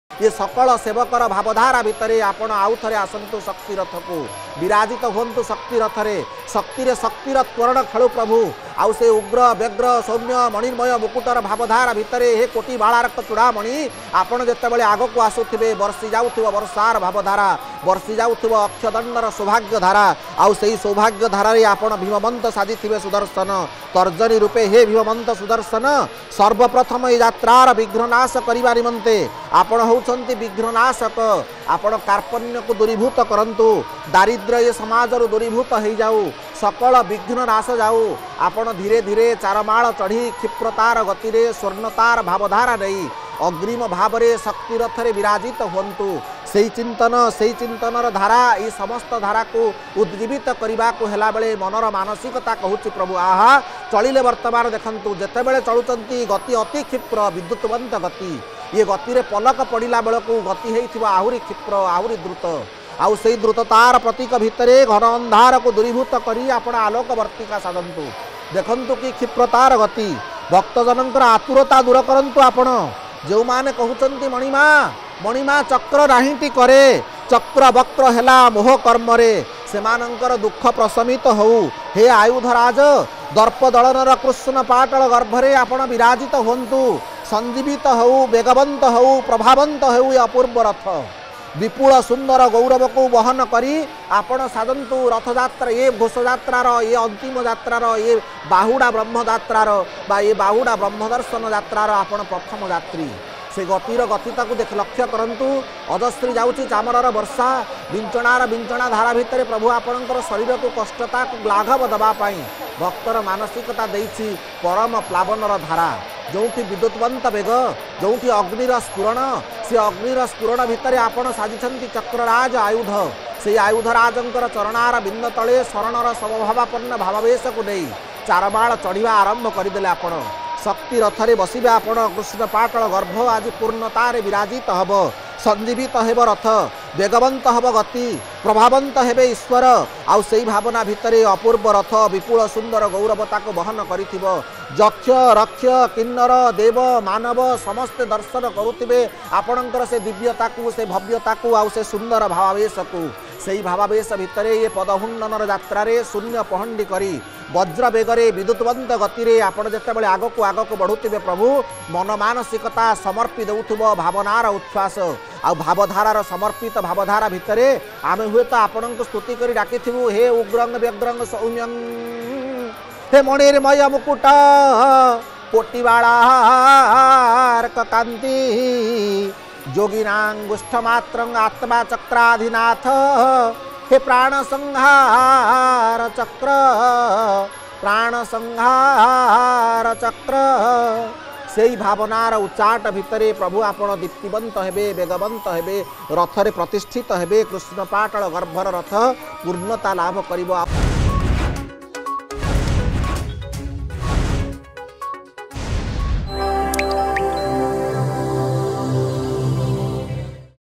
ରଥରେ ବିଜେ କଲେ ଶ୍ରୀ ସୁଦର୍ଶନ
ଚତୁର୍ଦ୍ଧାମୂର୍ତ୍ତିଙ୍କ ପହଣ୍ଡି। ରଥରେ ବିଜେ କଲେ ଶ୍ରୀ ସୁଦର୍ଶନ ।